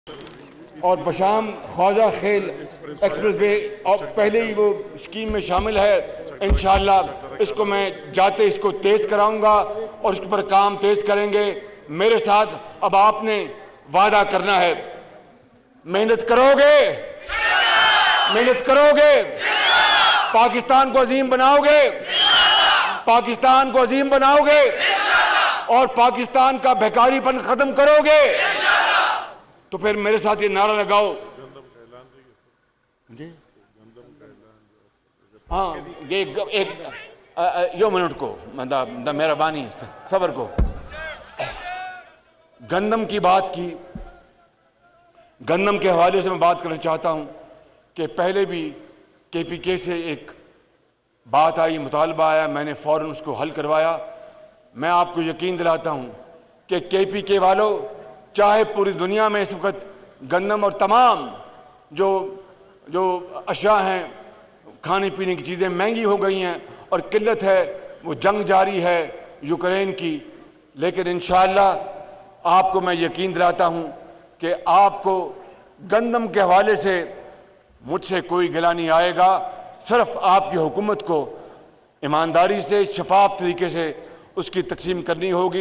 PM Shehbaz Sharif Ne Qaum Se wada Lay Liya
وزیر اعظم  نے جلسے کے دوران قوم سے وعدہ لے لیا!